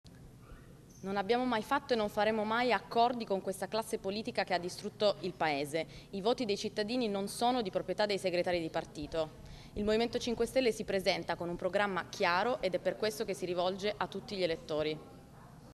Castelli (M5S): ballottaggi, mai accordi con classe politica che ha distrutto l'Italia
(Agenzia VISTA) - Roma, 9 Giugno 2016 - Le reazioni politiche dalla sala stampa di Montecitorio.